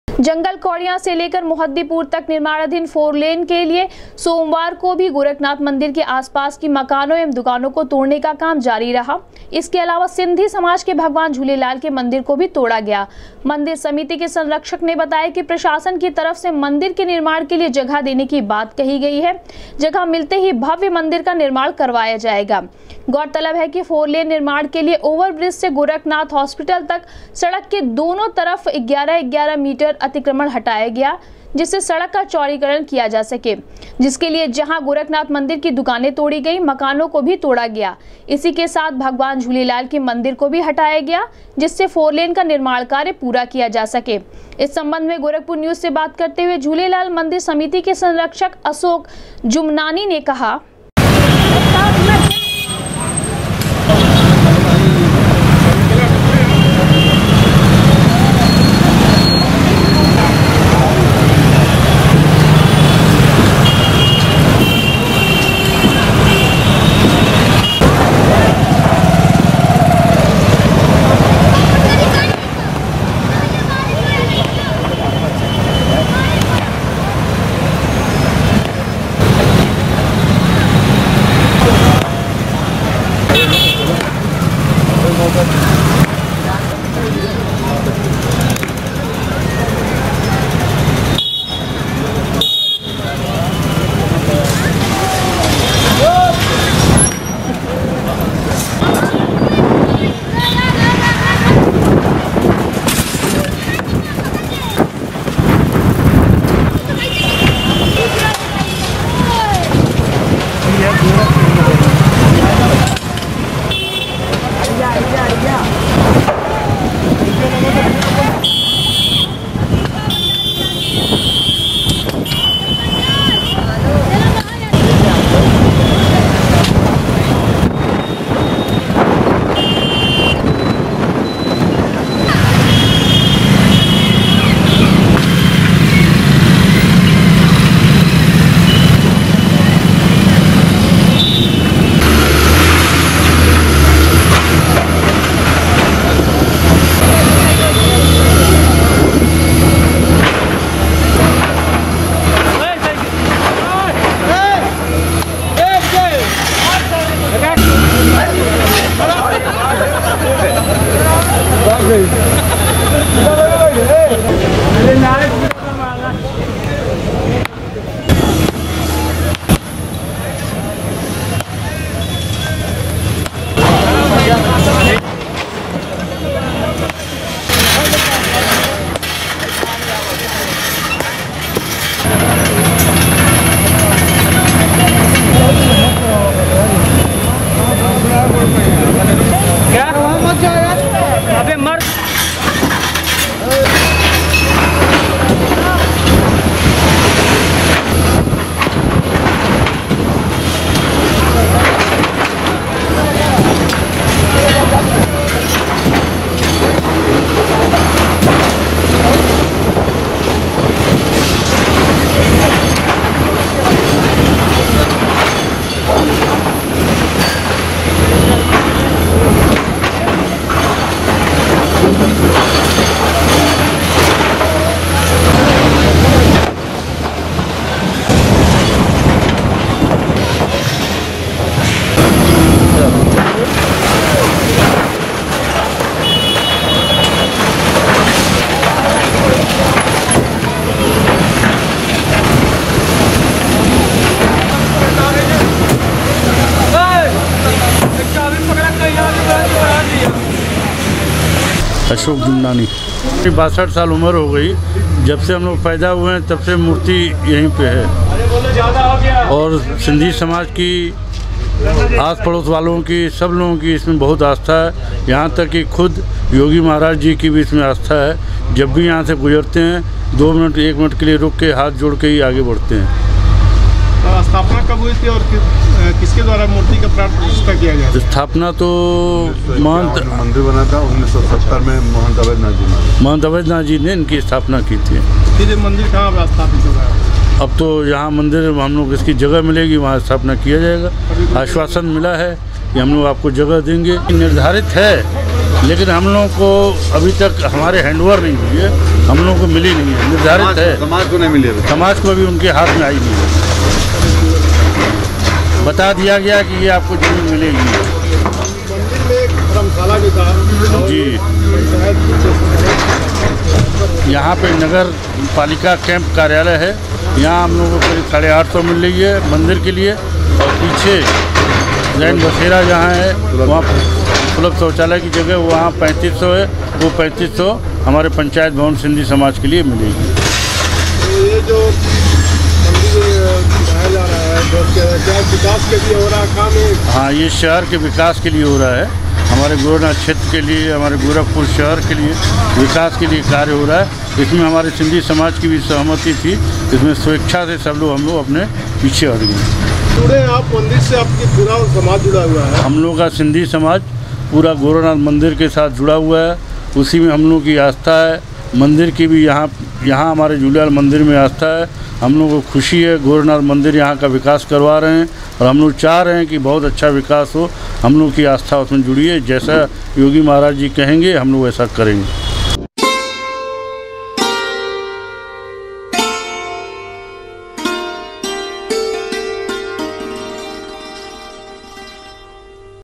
जंगल कौड़िया से मोहद्दीपुर तक बनाये जा रहे फोरलेन के लिए तोड़ा गया भगवान झूलेलाल का
जंगल कौड़िया से मोहद्दीपुर तक बनाये जा रहे फोरलेन के लिए तोड़ा गया भगवान झूलेलाल का मन्दिर ,मन्दिर समिति के संरक्षक ने कहा, विकास के लिए मुख्यमन्त्री योगी आदित्यनाथ के साथ है सिंधी समाज